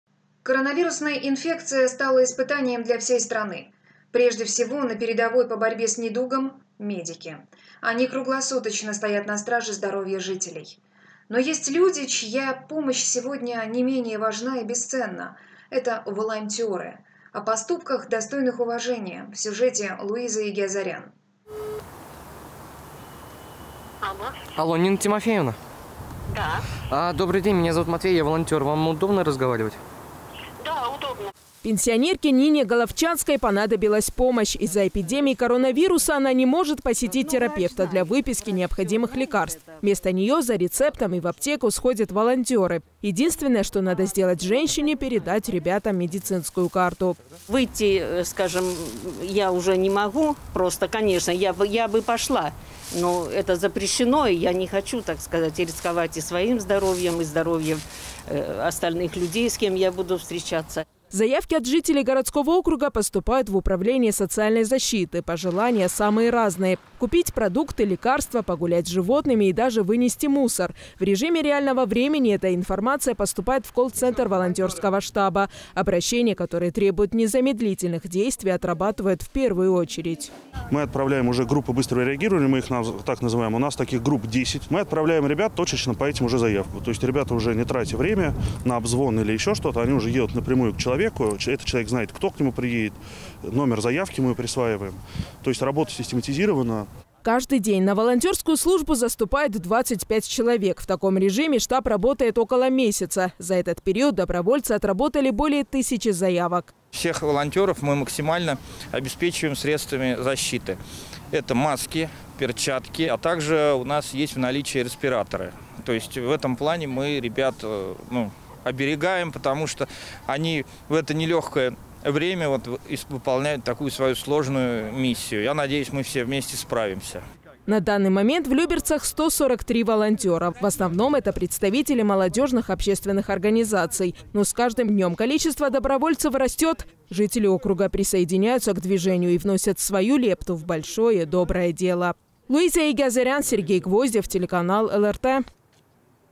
Помощь тем, кто в группе риска
Коронавирусная инфекция стала испытанием для всей страны. Прежде всего, на передовой по борьбе с недугом находятся медики. Они круглосуточно стоят на страже здоровья жителей. Но есть люди, чья помощь сегодня не менее важна и бесценна. Это – волонтеры. О поступках, достойных уважения – наш следующий сюжет.

"Алло, Нина Тимофеевна? Добрый день, меня зовут Матвей, я волонтер. Вам удобно разговаривать? Да, удобно, Матвей".

Пенсионерке Нине Головчанской понадобилась помощь. Из-за эпидемии коронавируса она не может посетить терапевта для выписки необходимых лекарств. Вместо нее за рецептом и в аптеку сходят волонтеры. Единственное, что надо сделать женщине – передать ребятам медицинскую карту.

Нина Головчанская, жительница г.о. Люберцы:
"Выйти я уже не могу, конечно, я бы пошла, но это запрещено. Я не хочу рисковать и своим здоровьем и здоровьем остальных людей, с кем я буду встречаться".

Заявки от жителей городского округа поступают в управление социальной защиты. Пожелания – самые разные. Купить продукты, лекарства, погулять с животными и даже вынести мусор. В режиме реального времени эта информация поступает в колл-центр волонтерского штаба. Обращения, которые требуют незамедлительных действий, отрабатывают в первую очередь.

Георгий Николаев, руководитель волонтерского штаба г.о. Люберцы:
"Мы отправляем группы быстрого реагирования, мы их так называем. У нас таких групп. Мы отправляем ребят точечно по этим заявкам. То есть, ребята, не тратя времени, уже едут напрямую к человеку. Этот  человек знает, кто к нему приедет, номер заявки мы присваиваем. То есть, работа систематизирована".

Каждый день на волонтерскую службу заступает 25 человек. В таком режиме штаб работает около месяца. За этот период добровольцы отработали более тысячи заявок.

Борис Новиков, начальник управления по работе с молодежью администрации г.о. Люберцы:
"Всех волонтеров мы максимально обеспечиваем средствами защиты. Это маски, перчатки, а также у нас есть в наличии респираторы. В этом плане мы ребят оберегаем, потому что они в это нелегкое время выполняют такую миссию. Я надеюсь, мы все вместе справимся".

На данный момент в Люберцах – 143 волонтера. В основном, это представители молодежных общественных организаций. Но с каждым днем количество добровольцев растет. Жители округа присоединяются к движению и вносят свою лепту в большое доброе дело.